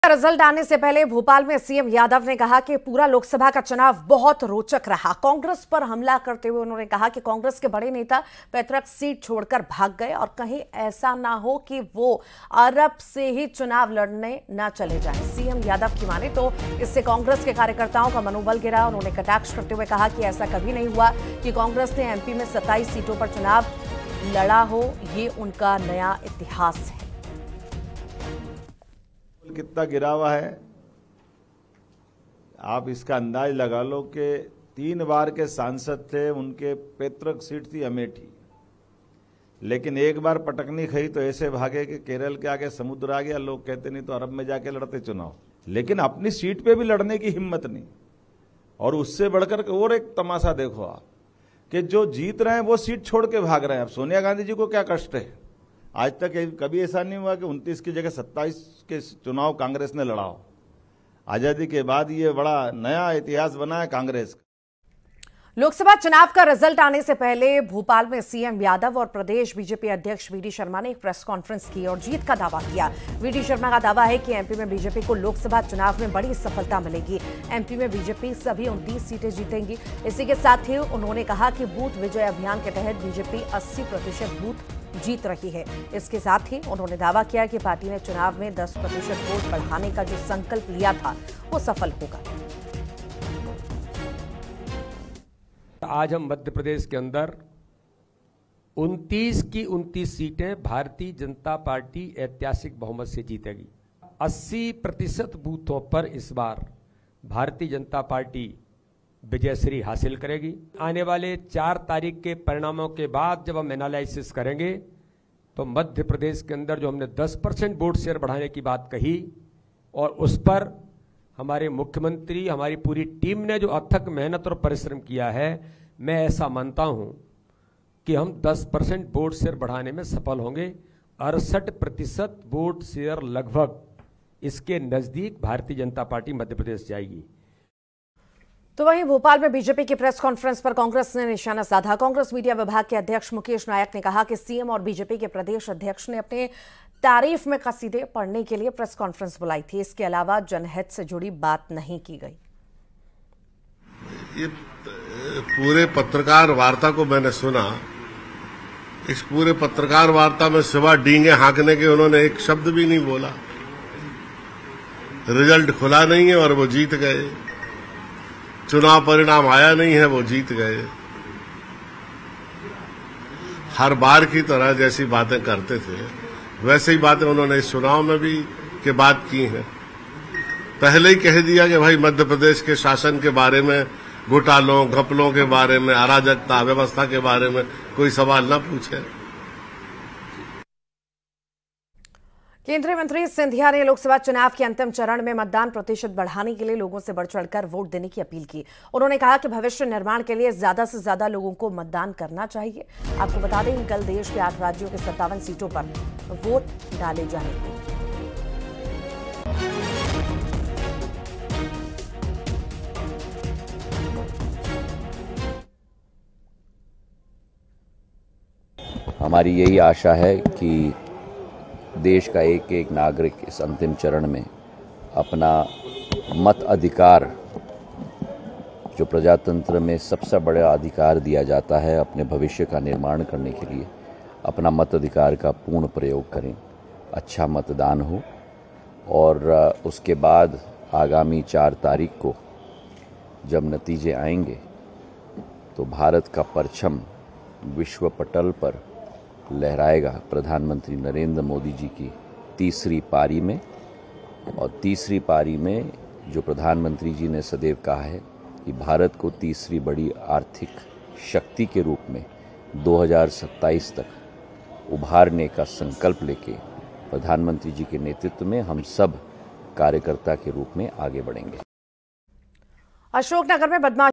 CM Mohan Yadav ने लोकसभा चुनाव पर कही बड़ी बात, कहा रहा रोमांचक | Breaking News | Lok Sabha Election
खबर है कि CM Mohan Yadav ने लोकसभा चुनाव पर कही बड़ी बात, कहा रहा रोमांचक, कांग्रेस पर भी जमकर बरसे